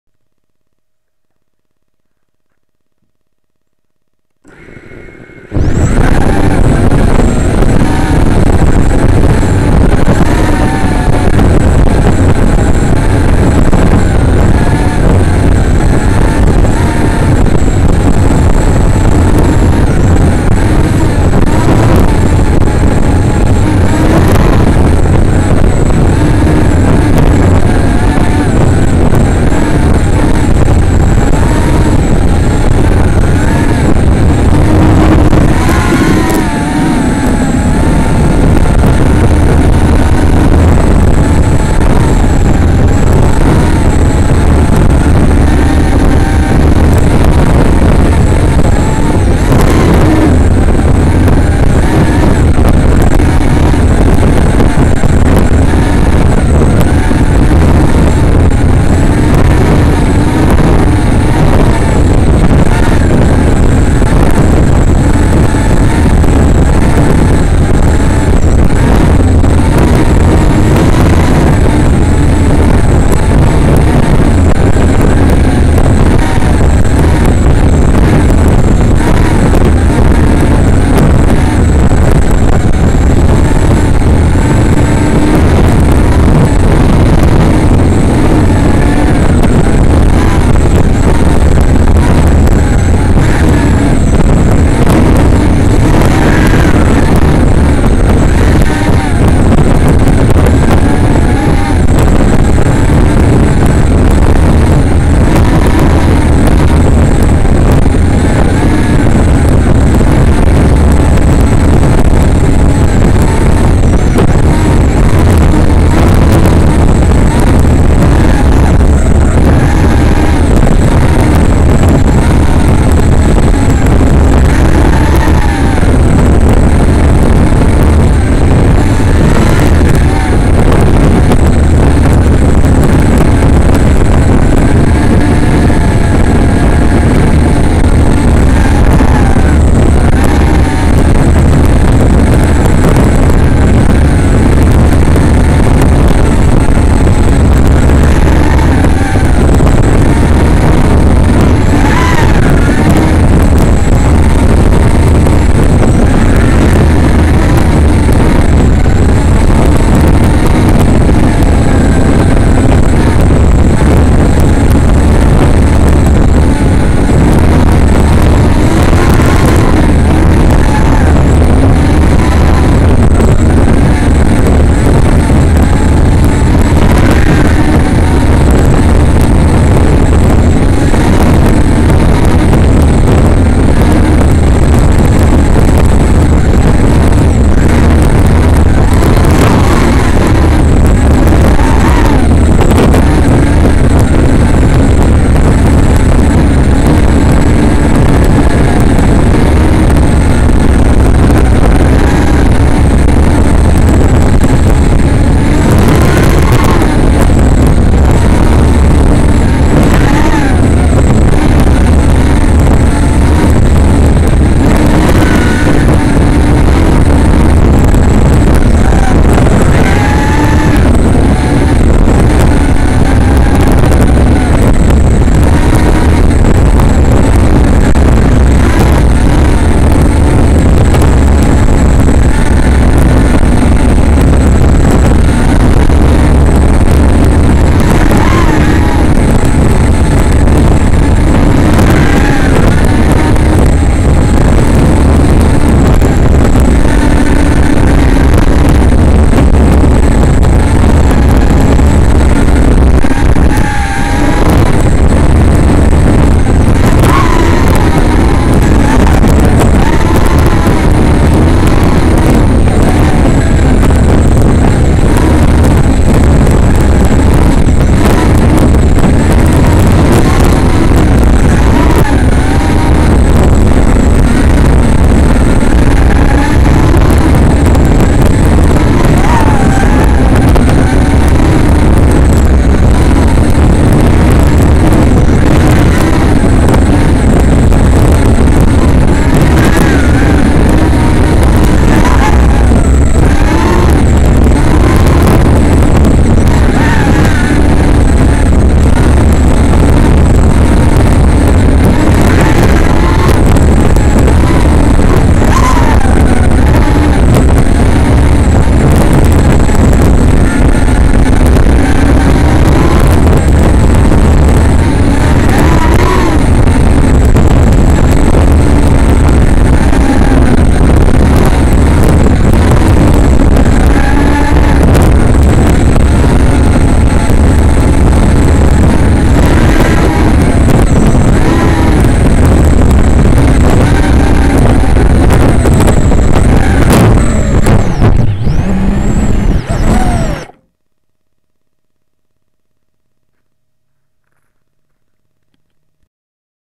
First Flight of The Day (Little Gap)
wanted to do some slow flying. but then decided to get a little risky. lol. thank God shes only a 3"